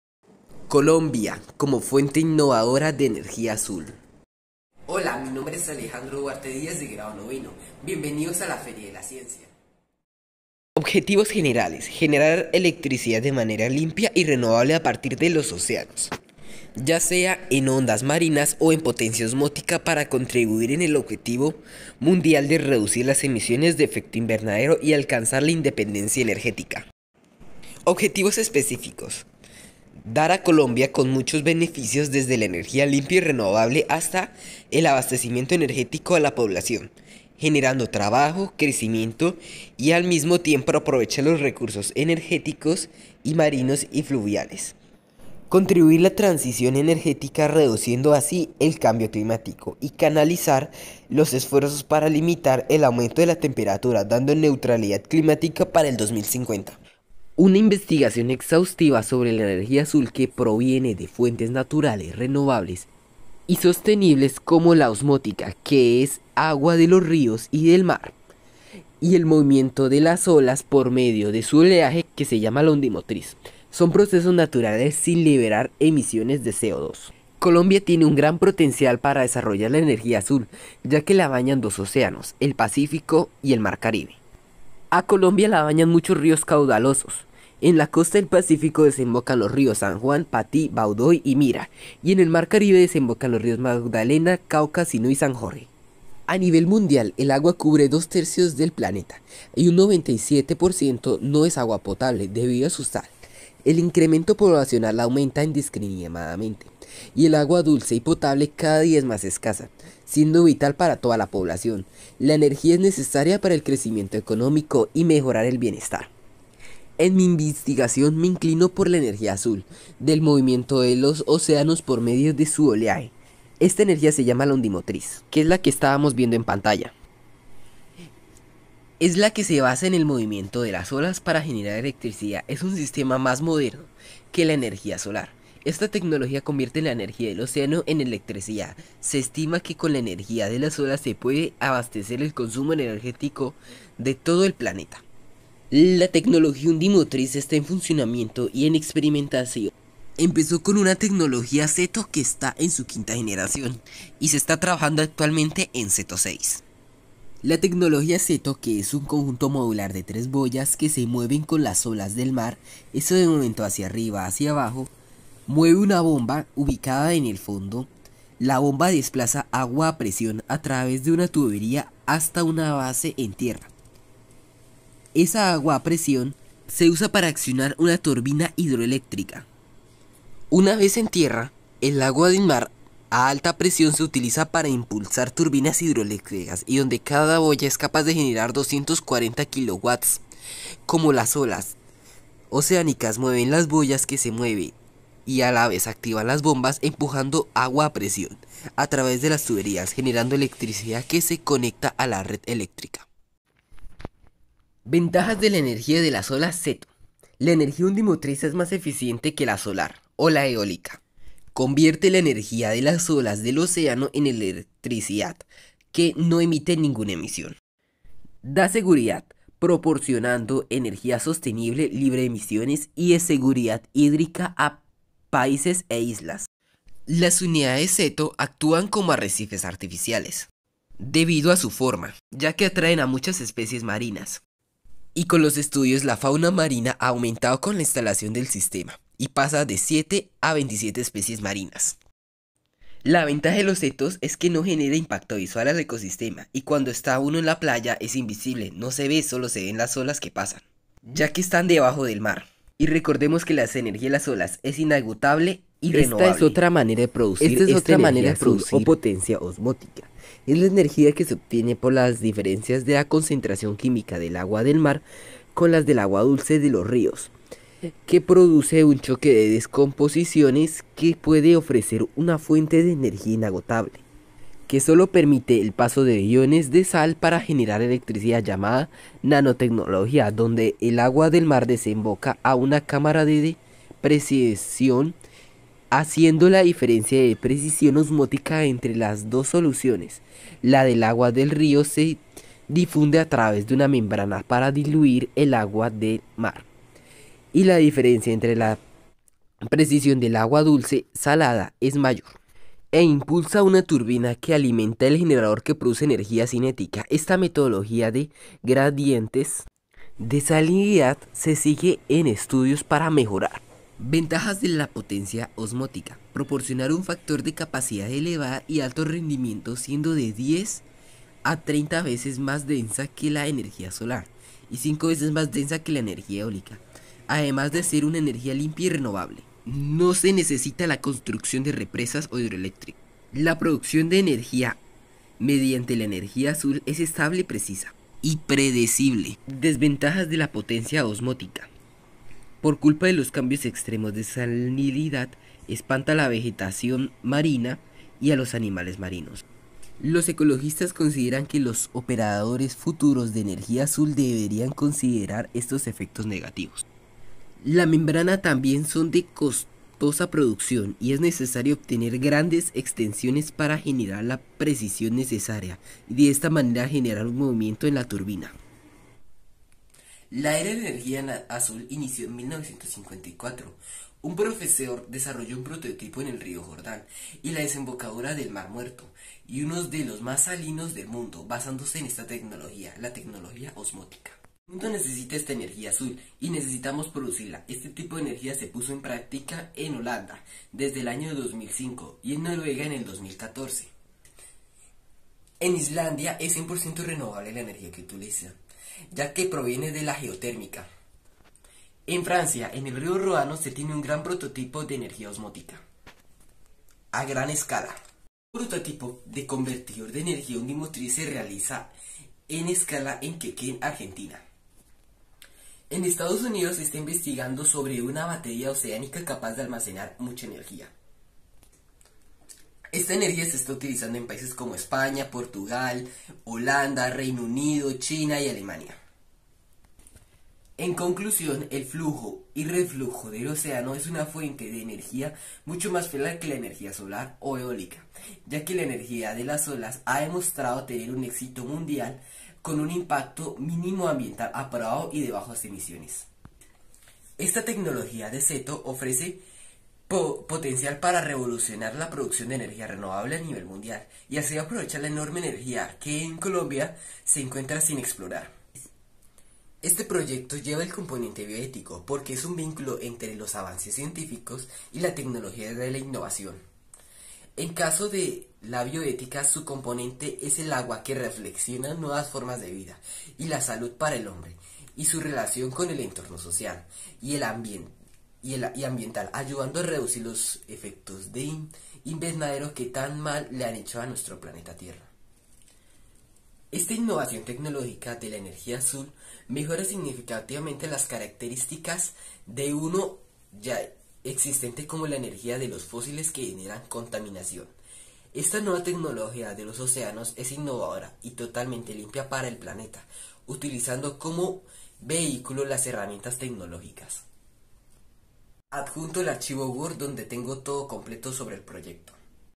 COLOMBIA COMO FUENTE INNOVADORA DE ENERGÍA AZUL
Proyecto día de la ciencia y tecnología
Alejandro Duarte Diaz
Grado: 9A